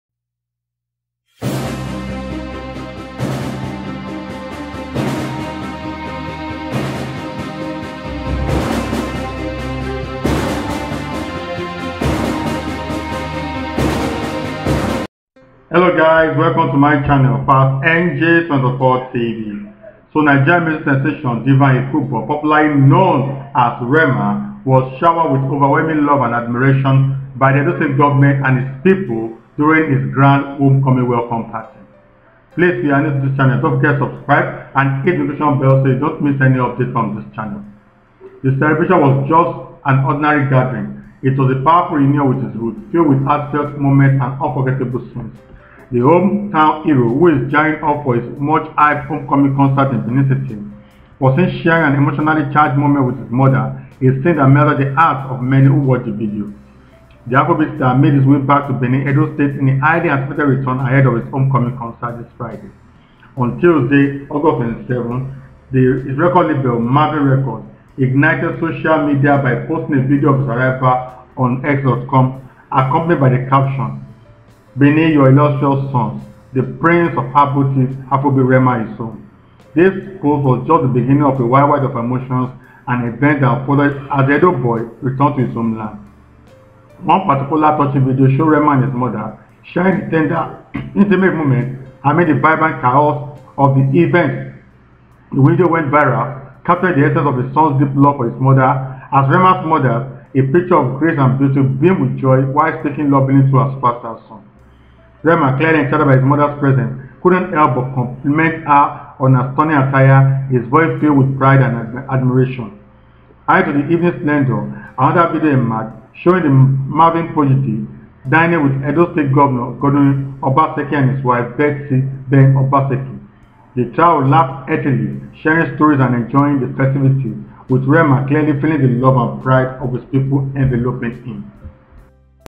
Moment Rema reunites with his mum, dines with Edo Governor, Obaseki and wife
Nigerian music sensation Divine Ikubor, known to the world as Rema, was showered with overwhelming love and admiration by the Edo State government and its people during his grand homecoming welcome party.